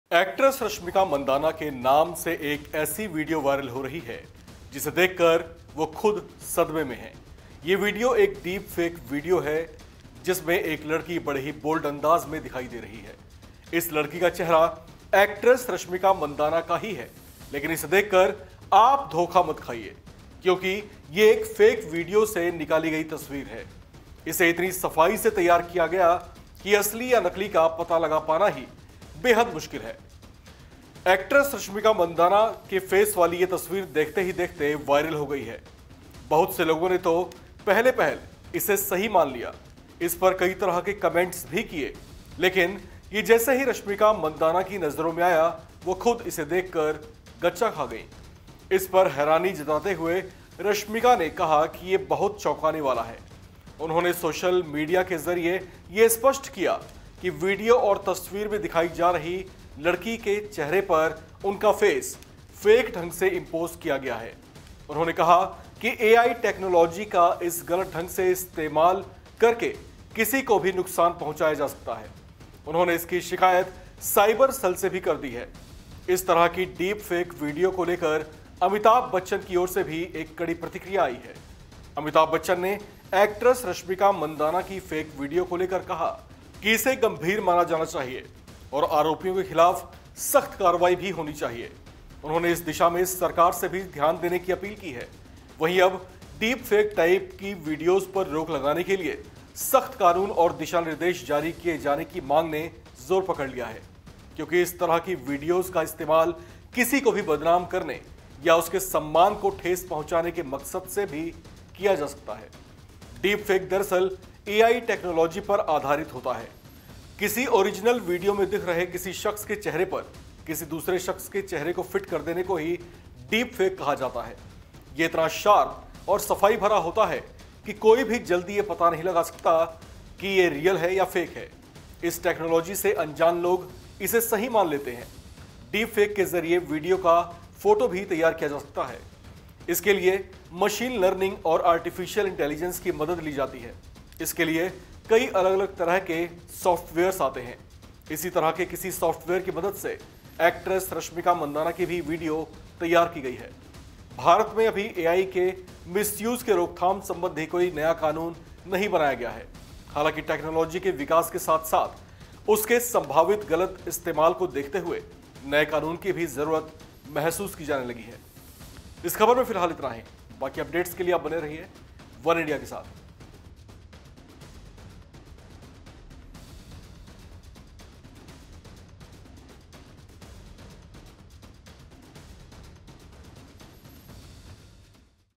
Rashmika Mandanna की Deepfake Video से मचा हड़कंप | Bollywood News | Entertainment | वनइंडिया हिंदी
Deepfake Video of Rashmika Mandanna: एक्ट्रेस रश्मिका मंदाना (Rashmika Mandanna) के नाम से एक ऐसी वीडियो वायरल (Viral Video) हो रही है, जिसे देखकर वो खुद सदमे मे हैं। ये वीडियो एक डीपफेक वीडियो (Deepfake Video) है, जिसमें एक लड़की बड़े ही बोल्ड अंदाज़ में दिखाई दे रही है। इस लड़की का चेहरा एक्ट्रेस रश्मिका मंदाना का ही है (Rashmika Mandanna Deepfake Video)। लेकिन इसे देखकर आप धोखा मत खाइये। क्योंकि ये एक फेक वीडियो से निकाली गई तस्वीर है। इसे इतनी सफाई से तैयार किया गया है, कि असली या नकली का पता लगा पाना भी बेहद मुश्किल है। एक्ट्रेस रश्मिका मंदाना (Actress Rashmika Mandanna) के फेस वाली ये तस्वीर देखते ही देखते वायरल हो गई है। लेकिन ये जैसे ही रश्मिका मंदाना की नज़रों में आया, वो खुद इसे देखकर गच्चा खा गईं। इस पर हैरानी जताते हुए रश्मिका ने कहा, कि ये बहुत चौंकाने वाला है। उन्होंने सोशल मीडिया के ज़रिए ये स्पष्ट किया कि वीडियो और तस्वीर में दिखाई जा रही लड़की के चहरे पर उनका फेस फेक ढंग से इंपोज़ किया गया है। उन्होंने कहा कि AI टेक्नोलॉजी का इस गलत ढंग से इस्तेमाल कर किसी को भी नुकसान पहुंचाया जा सकता है। इस तरह की डीपफेक वीडियो को लेकर अमिताभ बच्चन (Amitabh Bachchan) की ओर से भी एक कड़ी प्रतिक्रिया आई है। अमिताभ बच्चन ने एक्ट्रेस रश्मिका मंदाना की फेक वीडियो (Amitabh Bachchan on Rashmika Mandana Deepfake Video) को लेकर कहा, कि इसे गंभीर माना जाना चाहिए और आरोपियों के खिलाफ सख्त कार्रवाई भी होनी चाहिए।